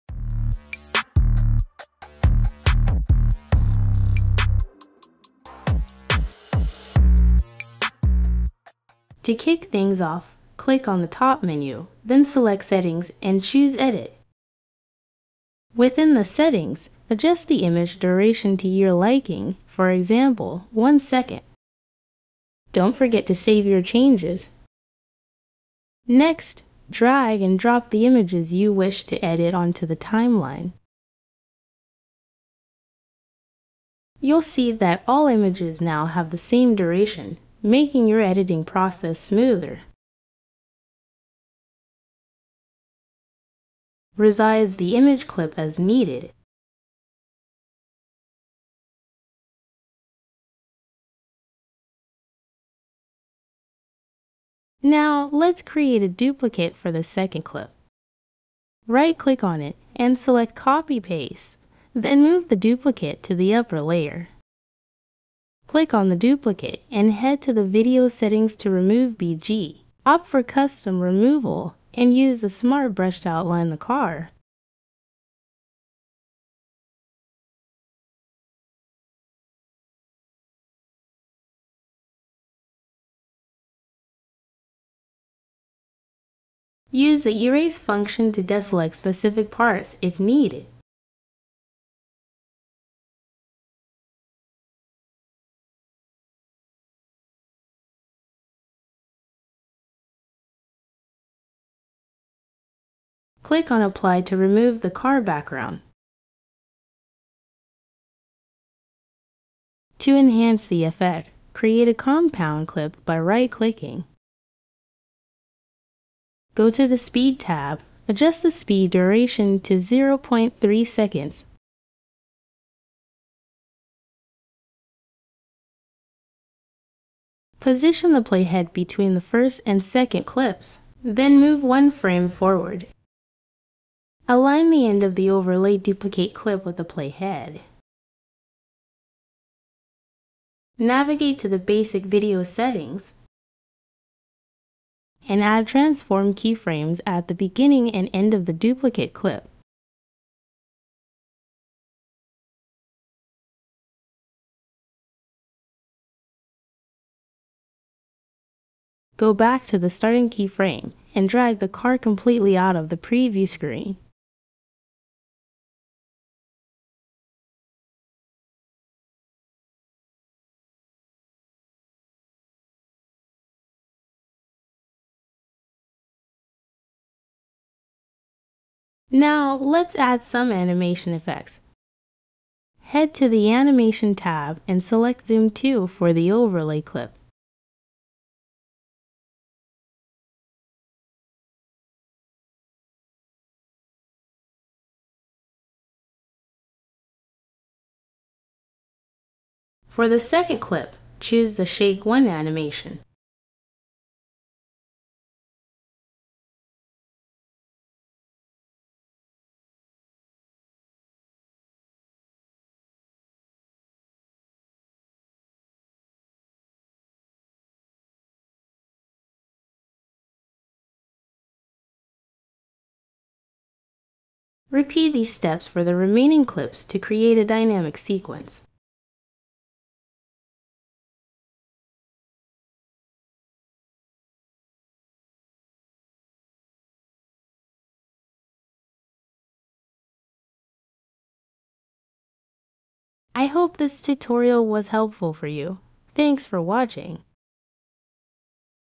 How to Create a Viral Car Slam Effect in CapCut PC (Pro)
Master the art of creating viral videos with the Car Slam Effect tutorial in CapCut PC!